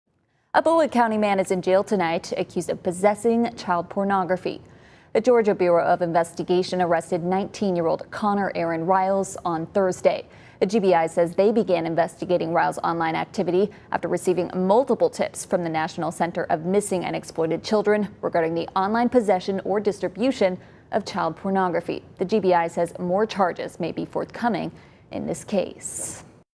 Bulloch County man arrested, accused of possessing child pornography
The Georgia Bureau of Investigation arrested 19-year-old Conner Aaron Ryles on Thursday.